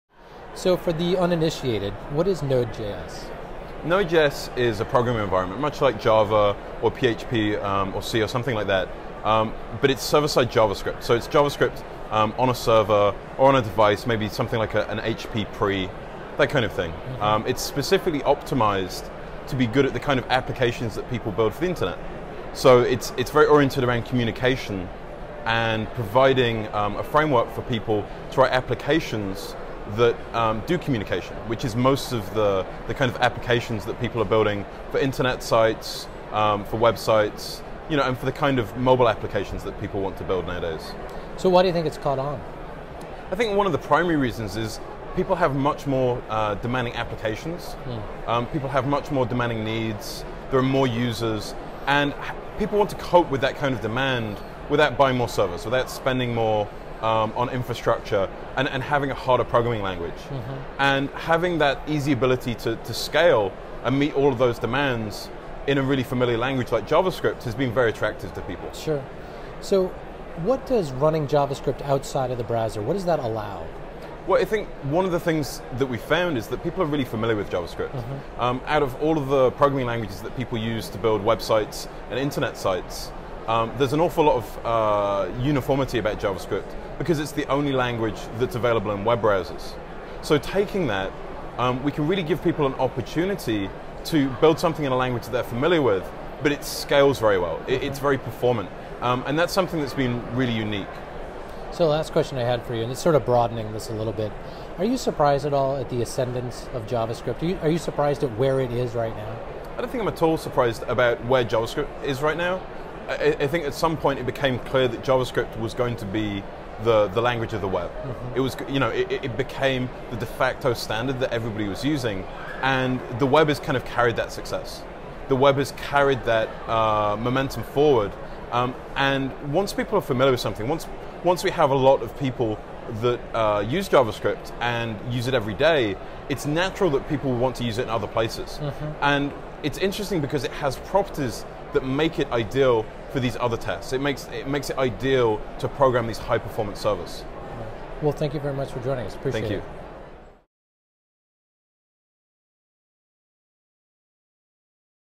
Tom Hughes-Croucher interviewed at Web 2.0 Expo San Francisco 2011
Tom Hughes-Croucher is the Chief Evangelist at Joyent. He provides technology leadership and outreach on behalf of the company. Tom is the author of the upcoming O'Reilly book "Up and Running with Node.js".

Before joining Joyent he was the Lead Technology Evangelist for North America and a Senior Developer at Yahoo!. Tom has contributed to a number of Web standards for the World Wide Web Consortium (W3C) and the British Standards Institute (BSI). He is worked with some of the world's leading brands including NASA, Yahoo!, Tesco, Three Telecom and the UK's Channel 4 Television.

Joyent is a cloud software start up specializing in low latency, high-performance data center software.